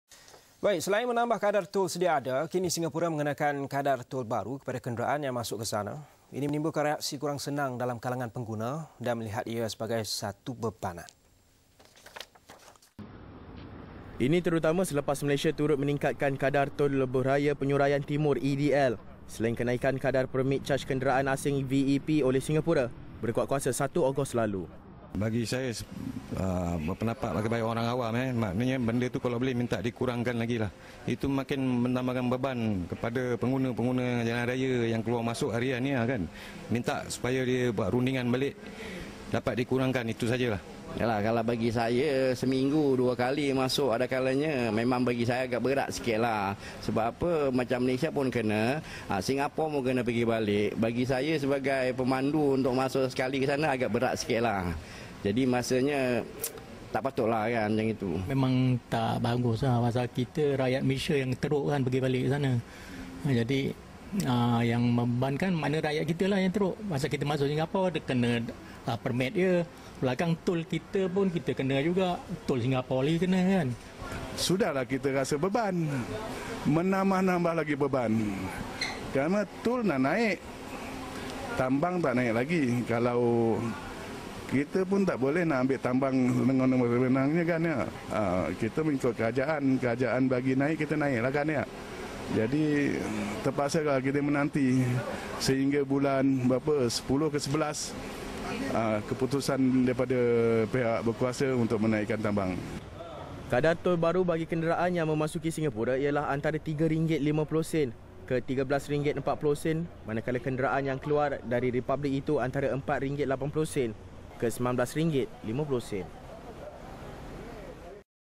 Kadar tol baru untuk masuk ke Singapura
Selain menambah kadar tol sedia ada, kini Singapura mengenakan kadar tol baru kepada kenderaan yang masuk ke republik itu. Ini menimbulkan reaksi kurang senang di kalangan pengguna dan melihat ia  sebagai satu bebanan.